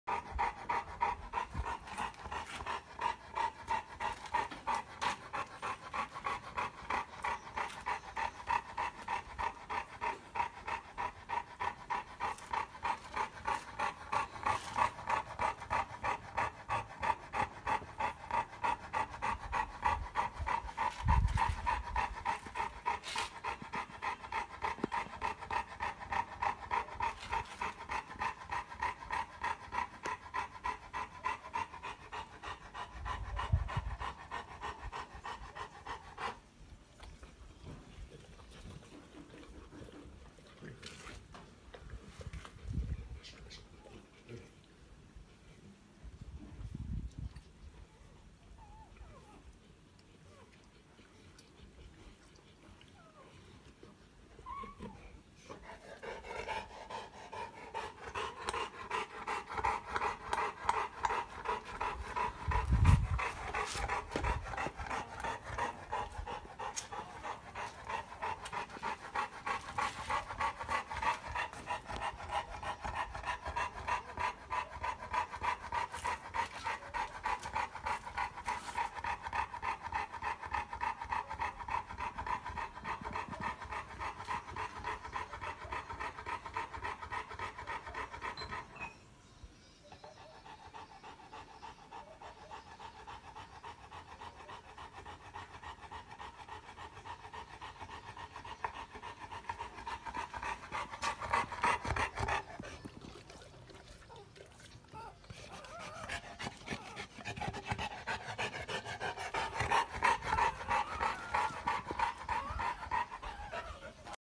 St Bernard Dog (Bella) giving birth to puppies
This is Bella, My St. Bernard dog giving birth to puppies. She already had two puppies and more are on the way.